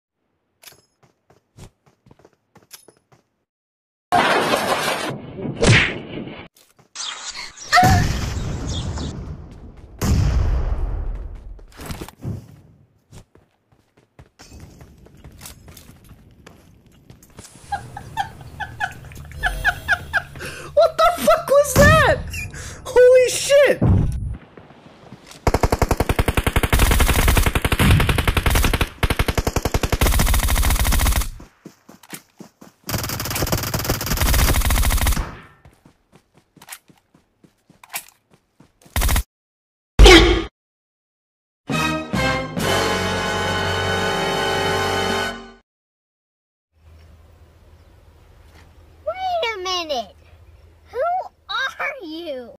RIP PLAYZONE 1 VS SQUAD ENDING 😑
tayangan ulang di akhir game " mati PLAYZONE gagal chicken